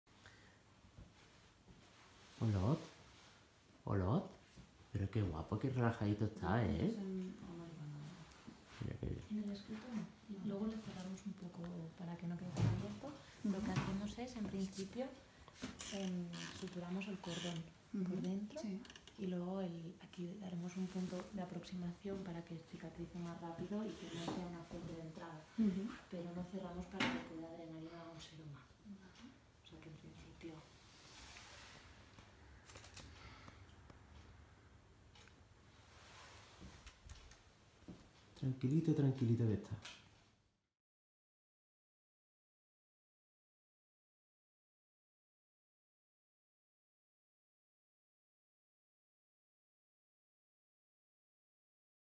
El cordero Olot ha sido esterilizado
Esta mañana hemos llevado a Olot a la clínica Girbet de Camprodon para esterilizarlo, y os hemos hecho este vídeo para que veáis lo tranquilo que está mientras se le realiza la intervención, porque confía plenamente en nosotros.

Olot fue encontrado en una montaña de Olot recién nacido a punto de morir, estaba solo, con hambre y muerto de frío. Conseguimos recuperarlo y salvarle la vida, y como podéis ver, cada día está más grande y guapo.